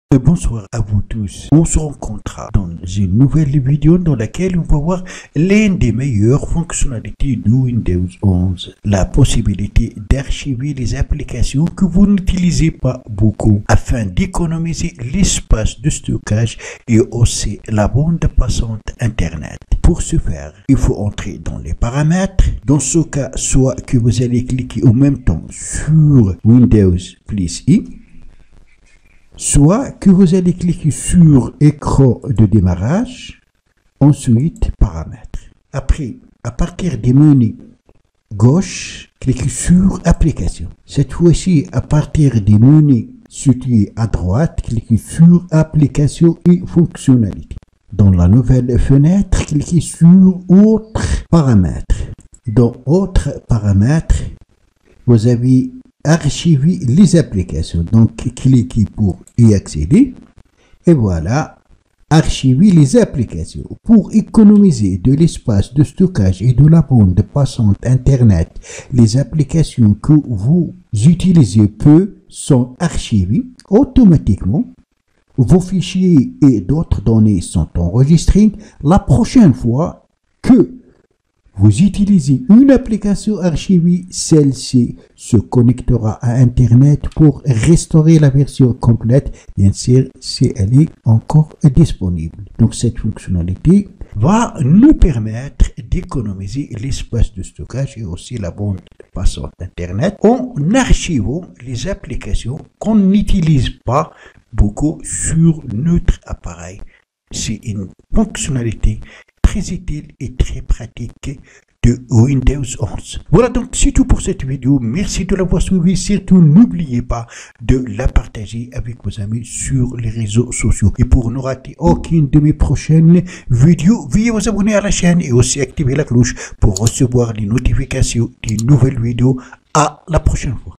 Windows 11 : Comment archiver les applications pour économiser l'espace de stockage .
L'une des meilleures nouvelles fonctionnalités de Windows 11 est la possibilité d'archiver les applications afin d'économiser l'espace de stockage et aussi la bande passante Internet, et la prochaine fois que vous utilisez une application archivée, celle-ci se connectera à internet pour restaurer la version complète ( si elle est disponible).